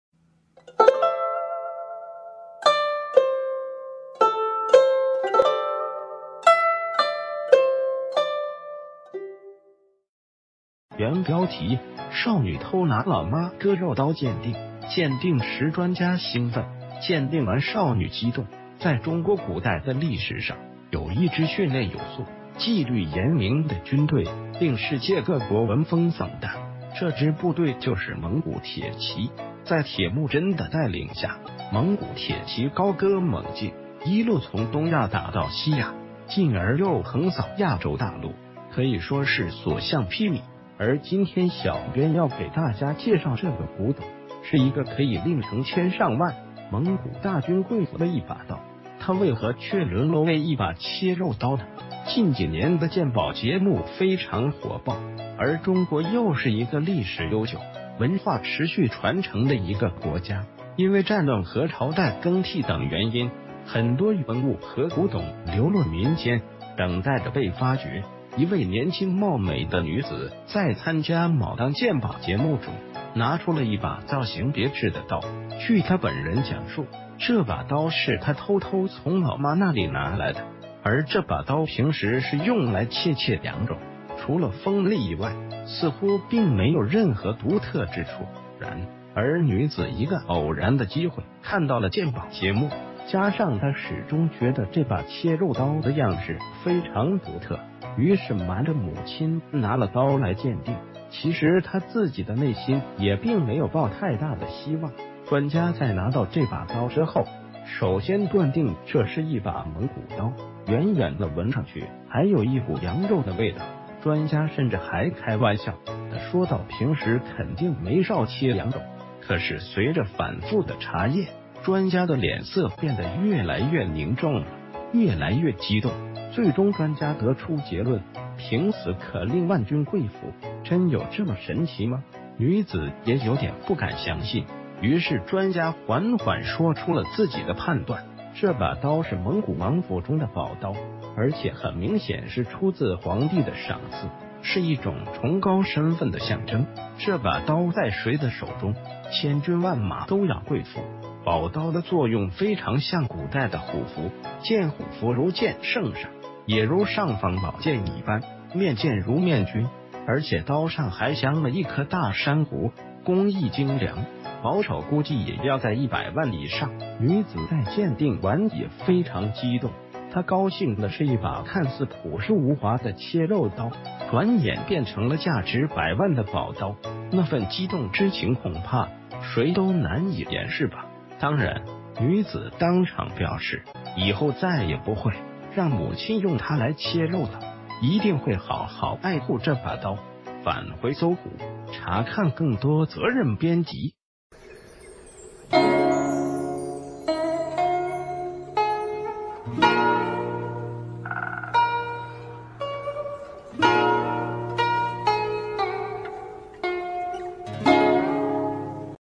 少女偷拿老媽 割肉刀鑒定，鑒定時專家興奮，鑒定完少女激動！_無名客棧歷史_無名客棧網
原標題：少女偷拿老媽...